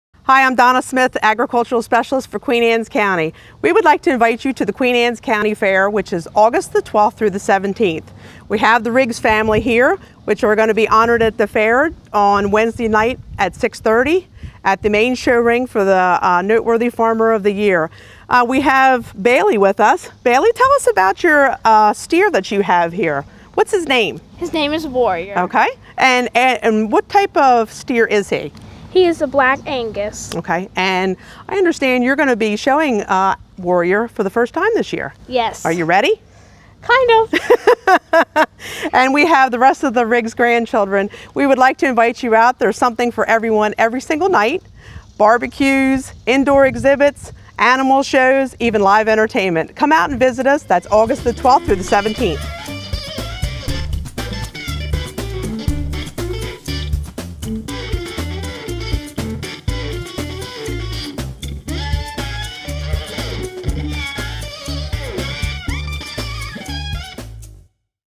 4H COMMERCIAL 2013 Riggs Farm Family
Come on out to the QAC 4H Fair! August 12-17. There's something for everyone!